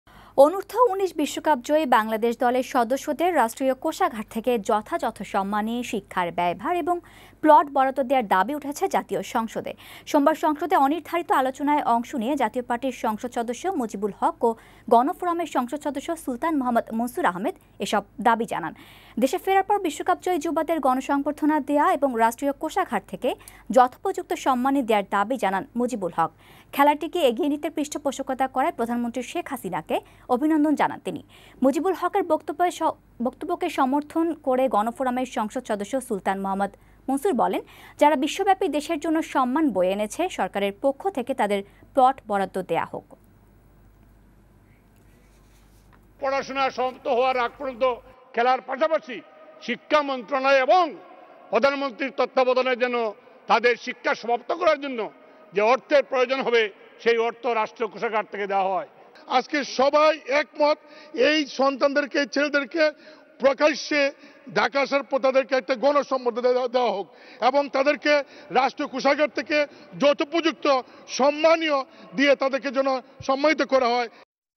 বিশ্বকাপজয়ীদের বিশেষ প্রনোদনা দেয়ার দাবি সংসদে
বিশ্বকাপজয়ীদের বিশেষ প্রনোদনা দেয়ার দাবি সংসদে | Parliament Members Demand | Joydeb | 10Feb20